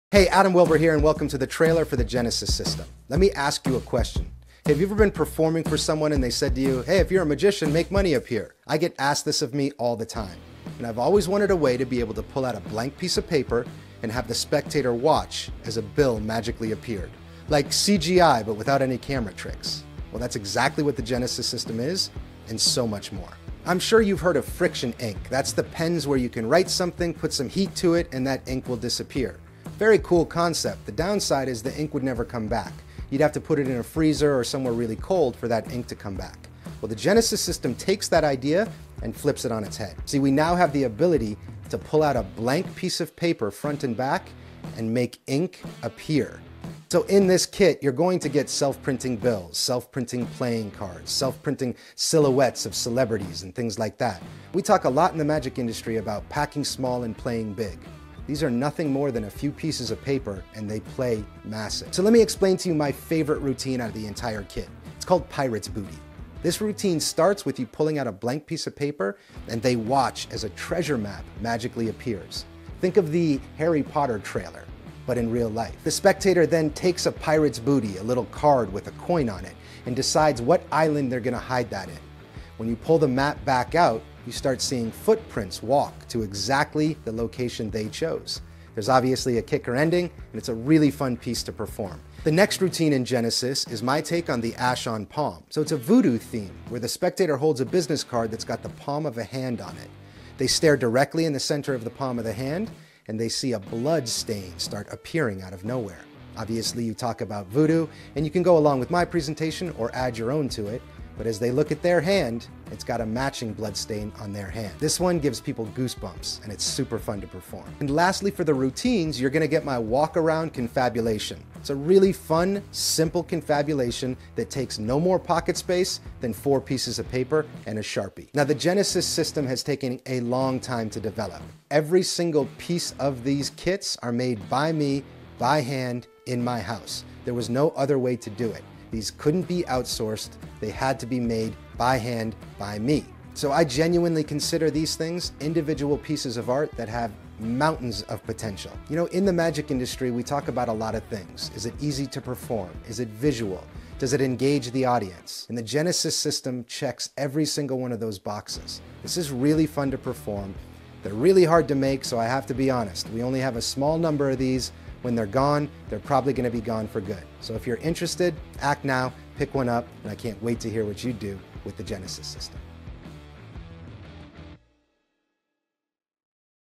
Genesis System Project by Adam Wilber and Vulpine Creations
The Genesis System is just that, a system. You get 33 hand-made gimmicks. Listed below are the routines and props included.

Blood Hand
Blood hand takes the ashes on palm plot to a level never seen before. Imagine giving your spectator a card with a hand-printed on it. As they stare at the hand, they notice a small drop of blood starts to appear. That blood drop slowly grows until it starts dripping down the wrist. You talk about voodoo and the idea that what happens to one thing must happen to another. The spectator turns their hand over to see a blood spot has shown up on their own palm. CRAZY spooky stuff.

If you've been a magician for any length of time, you've heard it before... "Can you make money appear?" Well now you have an answer to that question, and one that will blow them away! You pull out a blank sheet of paper and they watch as it slowly starts to materialize into existence. If ending clean is important to you, we go over a few easy options of how to do just that. This is absolute gold for "to camera" style of performance but also plays incredibly well in the real world, especially when someone asks you to make them some money with your magical powers

This is a beautiful piece that was designed to give people hope and allow them to believe their dreams will come true. You show someone a picture of a cherry blossom tree that they hold in their hands as they make a wish towards the tree. You let them know that if they believe in their heart that this wish will come true then the tree should blossom for them and in turn, the wish come true. Magic should be about helping other people feel better...this trick does just that!

Pirates Booty
This trick rocks! We combined magically appearing maps with a treasure hunt where the spectator gets to hide the treasure anywhere, they choose. Only to be revealed by the map itself in a super visual and stunning manner. The kicker ending happens when they realize the treasure itself has vanished and ended up on the chosen island in an impossible fashion. You get a lot of bang from such a small pocket space footprint!

Celebrity Silhouettes
You show three blank cards on the front and back, place them into three spectators' hands as they pick from a list of celebrities. Once they have decided on their celeb, you ask them to turn over the cards they have been holding the entire time. They notice the cards now contain a silhouette of the thought of person. There are a bunch of ways to present this trick and we go over multiple ones in the tutorial.

CLUE
Your spectator gets to write their own murder mystery case by picking the suspect and the murder weapon. This is a really fun and relatable effect that people simply love to be part of. Everyone knows the game of Clue, but nobody's seen it like this before. **You will need the game Clue for this one bonus routine.**

Playing Cards
You'll receive 6 black-faced cards, 9 red-faced cards, and 3 gaff cards as well as 2 red back designs and 2 Blue back designs. The red cards come with a royal flush that allows for some super fun gambling presentations.

Strolling Confabulation
In this confabulation routine, everything you need fits nicely into one small pocket of your jacket or pants. You place a blank prediction on the table and allow your spectators to choose their location, celebrity date, means of transportation, and preferred drink. Once these items have been chosen and agreed upon, the prediction is turned over to show it somehow magically printed itself into the exact scene picked by the audience members. A full multi-person confabulation in the palm of your hand.
Available from Magicbox